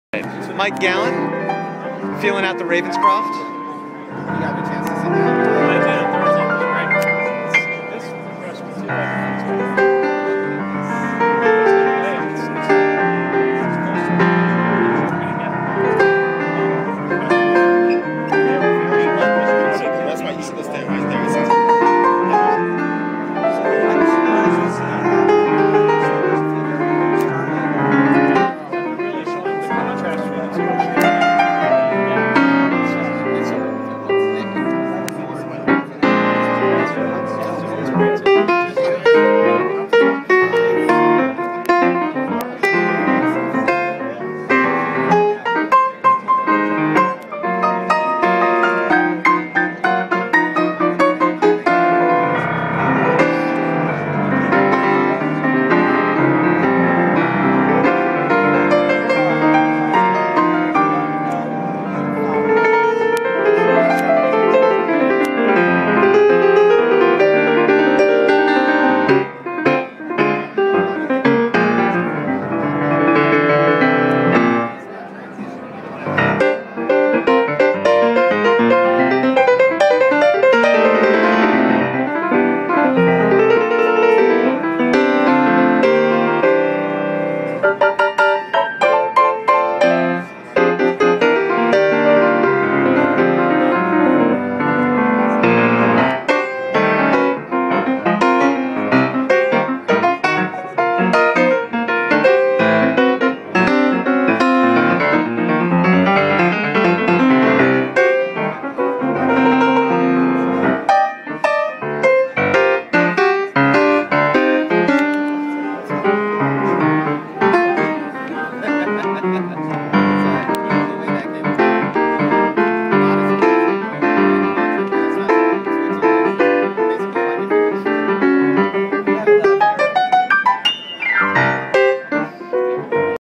NAMM 2014 Michael Gallant at Ravenscroft Pianos. Video by Keyboard Magazine
Michael Gallant of Keyboard Magazine gets to experience the magnificent Ravenscroft Model 275 9' concert Grand Piano at NAMM 2014.  The CAD optimized action is the best and smoothest in the industry.